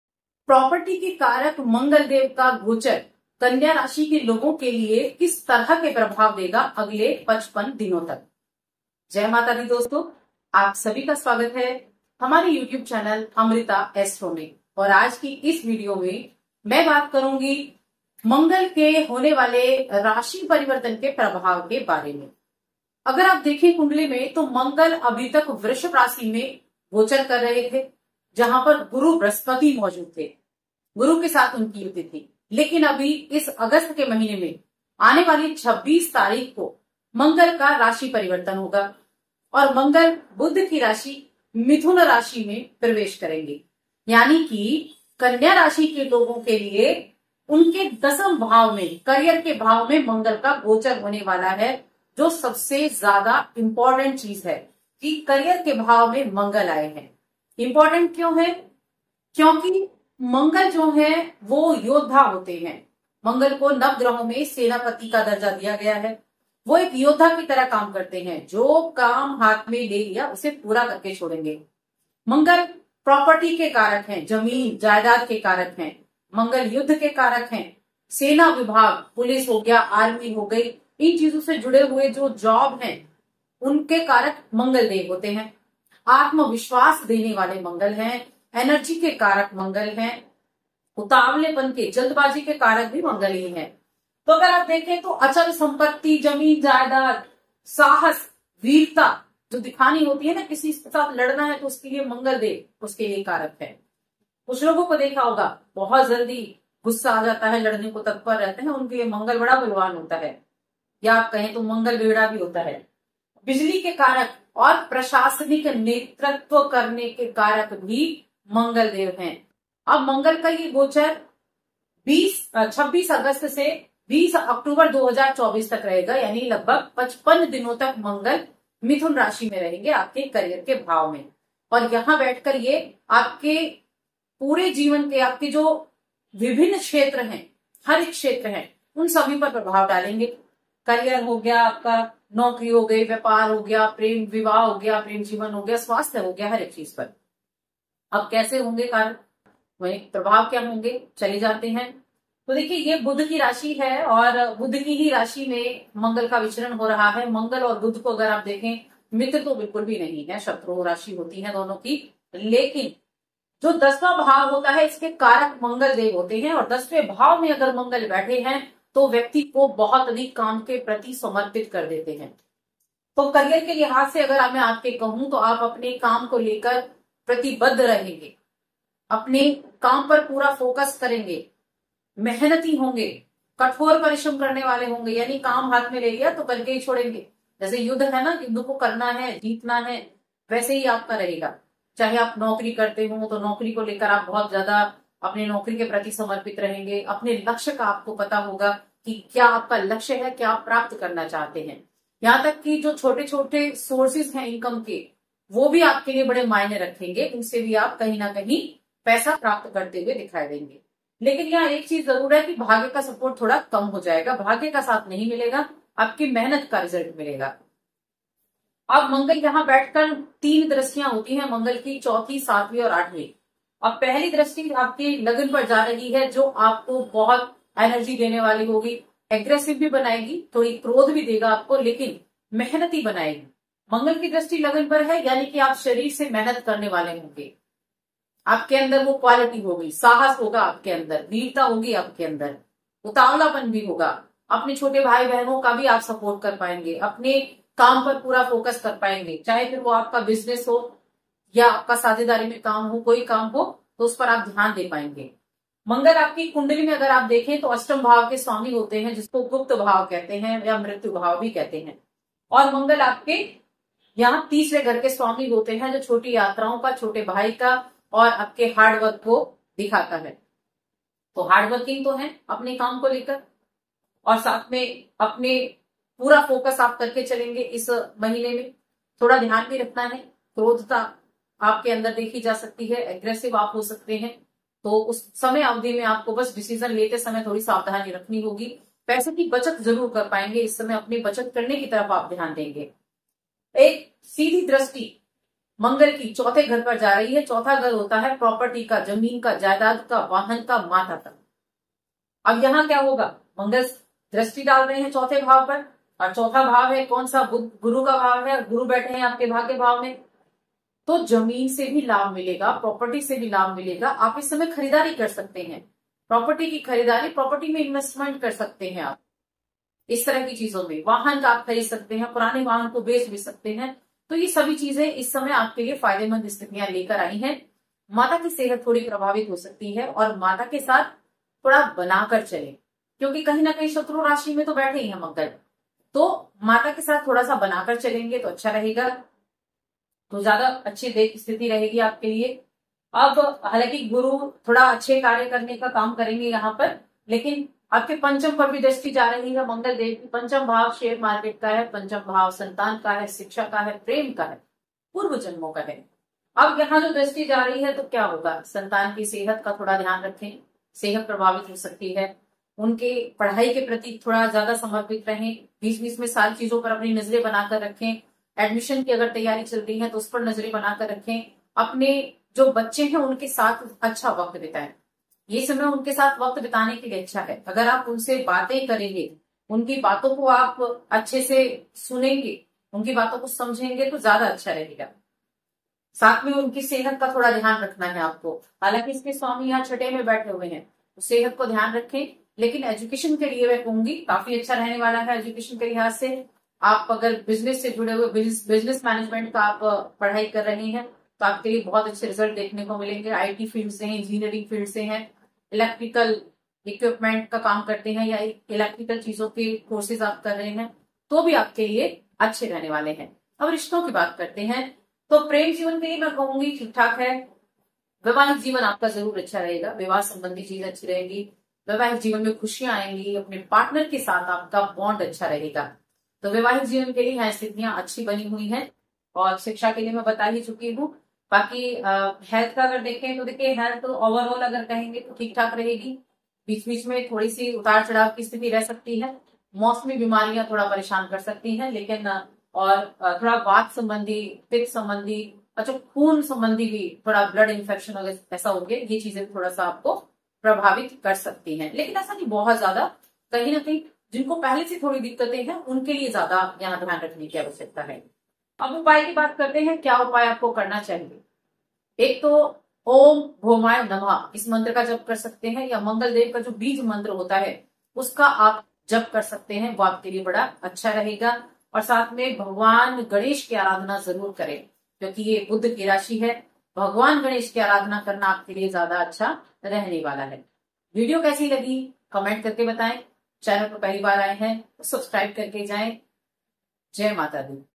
कन्या राशि मंगल गोचर से 55 दिन अच्छा मुनाफा मिलेगा । 26 अगस्त मंगल गोचर। कन्या राशिफल
कन्या राशि मंगल गोचर से 55 दिन अच्छा मुनाफा मिलेगा । 26 अगस्त मंगल गोचर। कन्या Rashifal